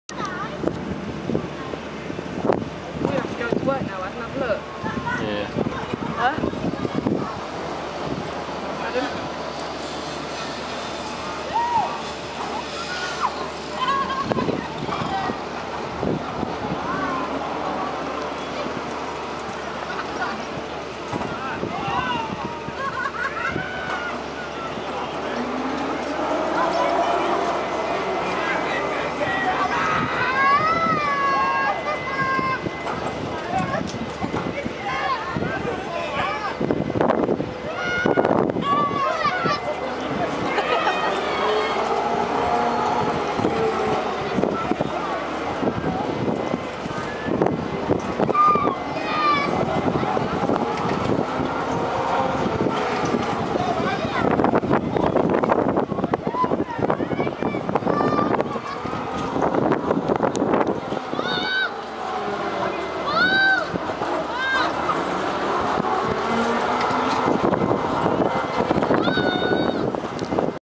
Sky Flyer at Flamingo Land
Sky Flyer double inverted ship at Flamingo Land. This ride has since been removed and ended up at Pleasure Island in Cleethorpes. Now that has closed the location of this ride is currently unknown.